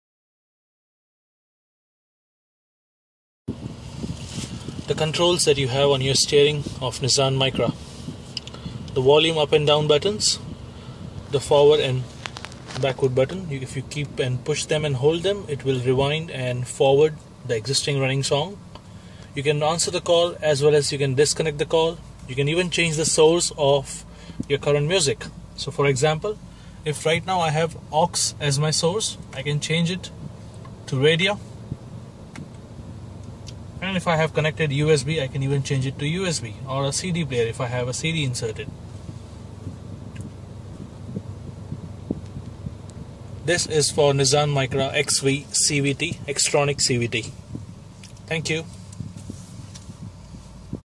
Nissan Micra XV CVT - Controls on Steering
A quick video demonstrating controls available on Nissan Micra XV CVT.